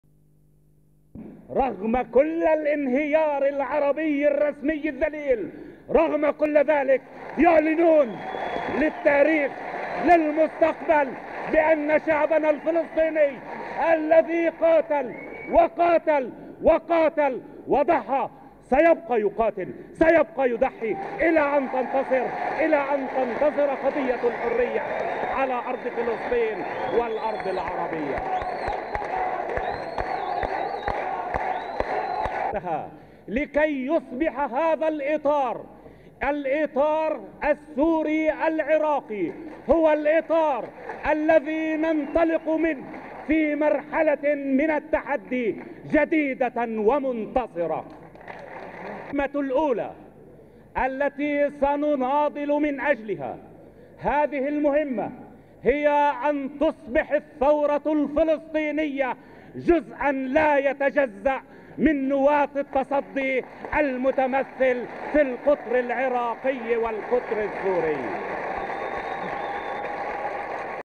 SYND 2 11 78 HABASH SPEAKS AT POPULAR FRONT FOR LIBERATION OF PALESTINE RALLY
(31 Oct 1978) The leader of the Popular Front For The Liberation Of Palestine, (PFLP), George Habash praised The Syrian-Iraqi alliance on Tuesday, (31 October), at a rally held in Beirut in favour of the alliance. He called on the palestinians to join forces with this alliance and to continue to "struggle for the palestinian revolution."